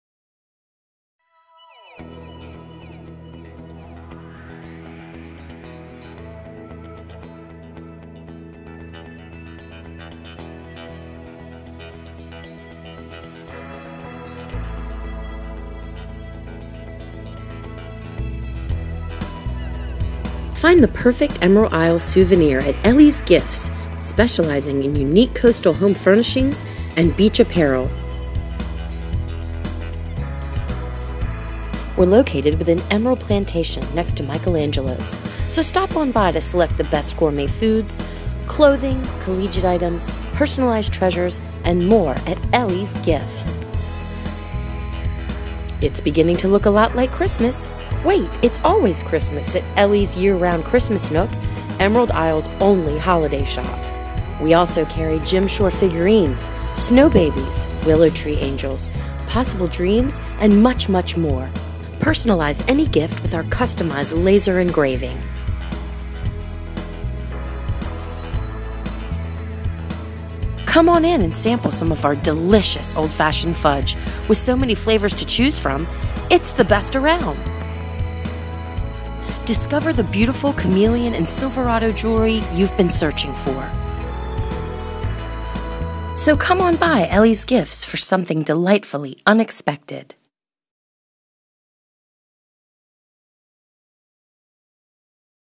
CC Elly's Gifts. Shopping in Emerald Isle, NC.
Elly's Gifts offers residents and visitors an amazing variety of products including home decor, gourmet foods, souvenirs, jewelry, collectibles, greeting cards, outdoor entertaining items and the best homemade fudge in the Crystal Coast. In addition, Elly's is home to Emerald Isle's only Christmas shop, featuring 12 beautifully decorated trees depicting a wide array of themes. Including personalized laser engraved gifts. We carry top quality brands such as Jim Shore, Willow Tree, Baggallini, Kameleon, Wind Willow and many more.

Video produced by VistaGraphics, Inc.